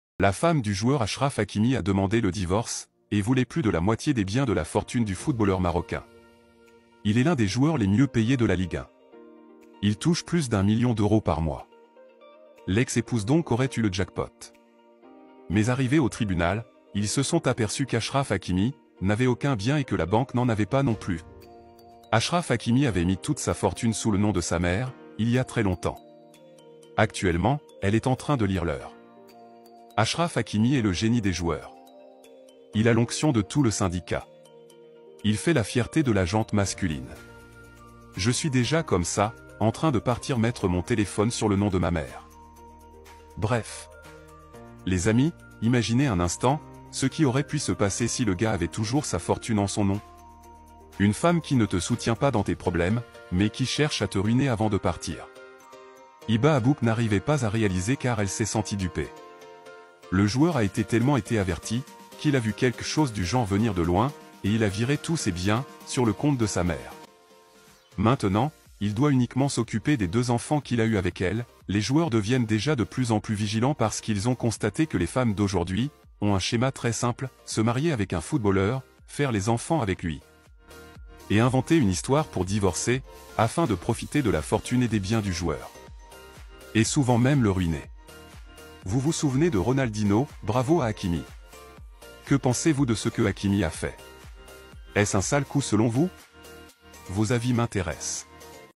Hakimi achraf || La femme de Hakimi ne s'attendait pas à ça, Mbappé, Meghan Markle,Nabille, cr7
Achraf Hakimi, le footballeur, a été mis en examen pour viol.

Une nouvelle qui a bouleversé sa femme. Elle a donc demandé le divorce et voulait plus de la moitié des biens et de la fortune du footballeur.

Il est l’un des joueurs les mieux payés de la ligue 1, il touche plus d’un million d’euros par mois.

Mais arrivés au tribunal, ils se sont aperçus qu’Achraf Hakimi n’avait aucun bien et que la banque n’en avait pas non plus.